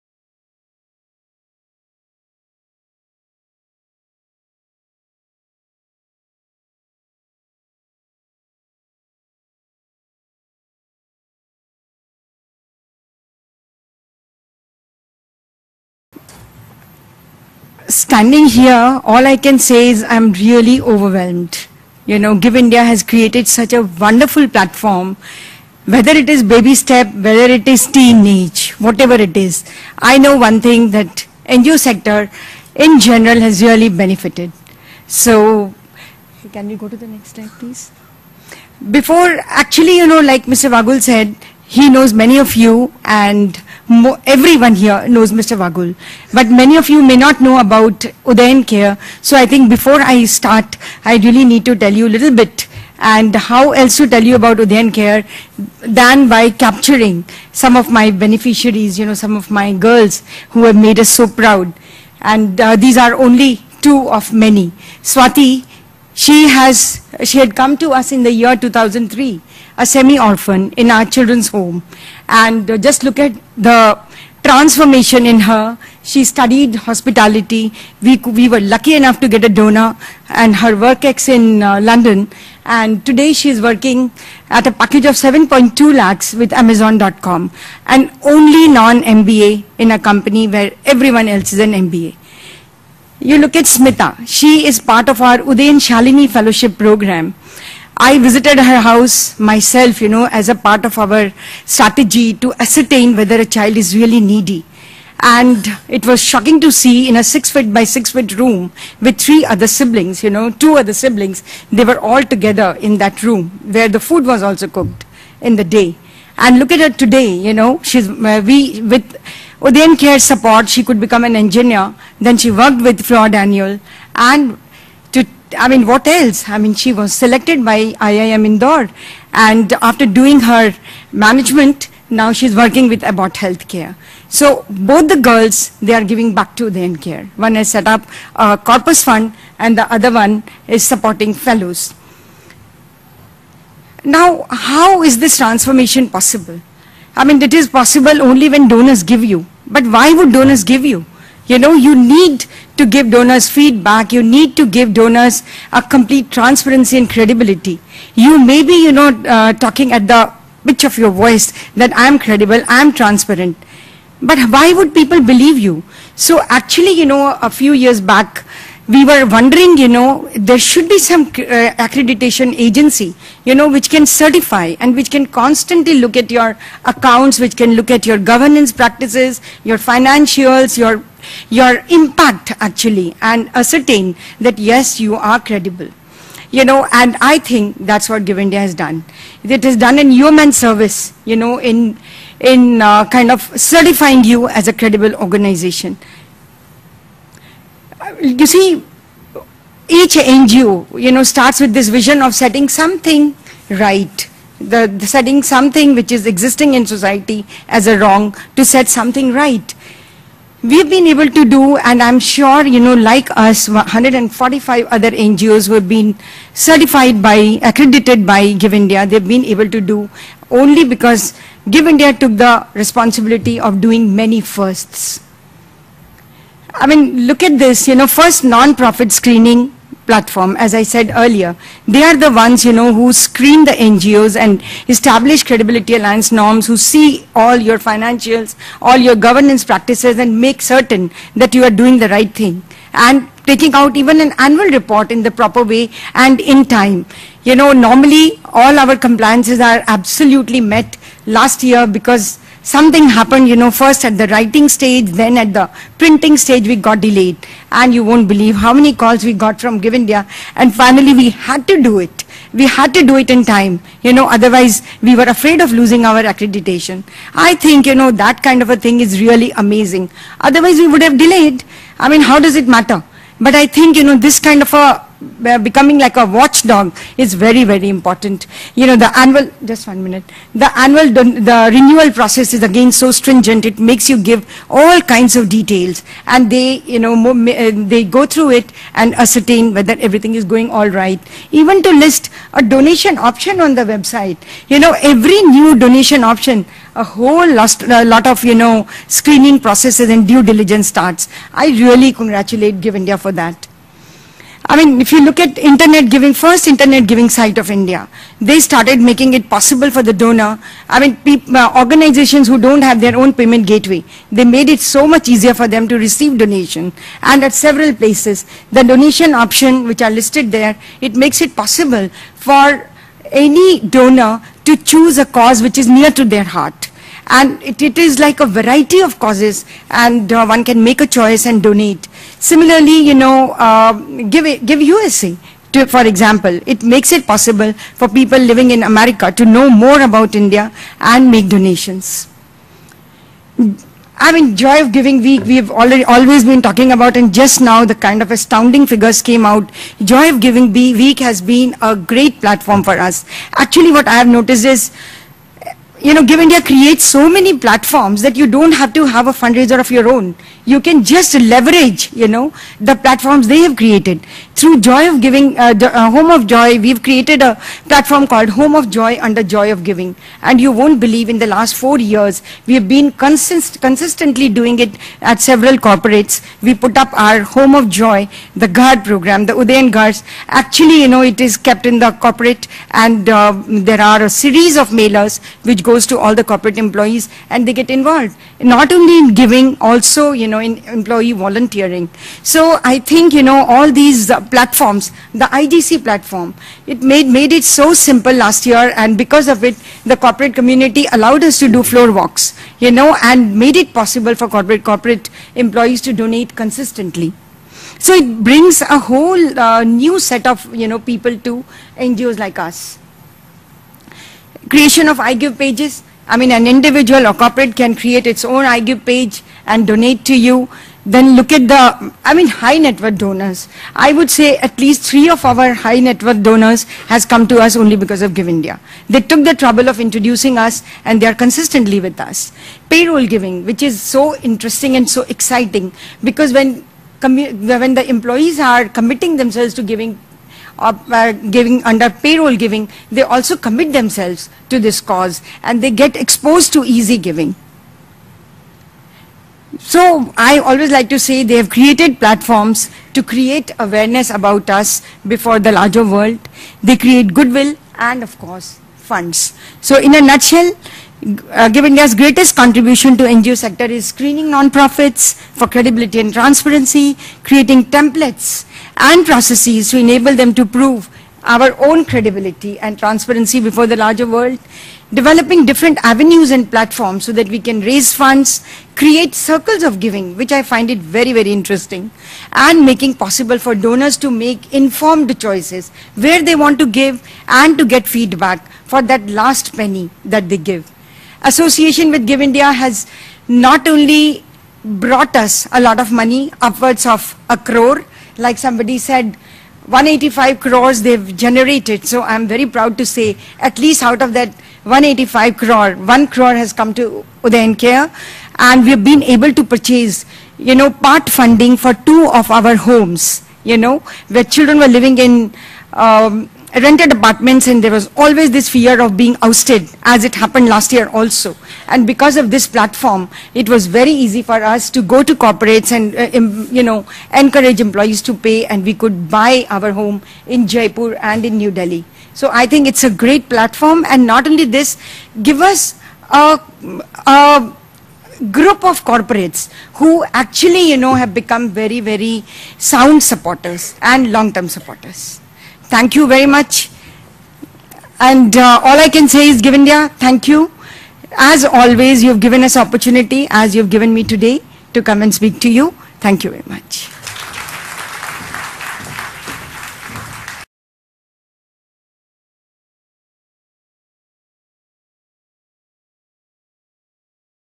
Kiran Modi at GiveIndia's 13th Anniversary celebration
Kiran Modi - Managing Trustee, Udayan Care, talks about GiveIndia's impact on the growth and development of Udayan Care.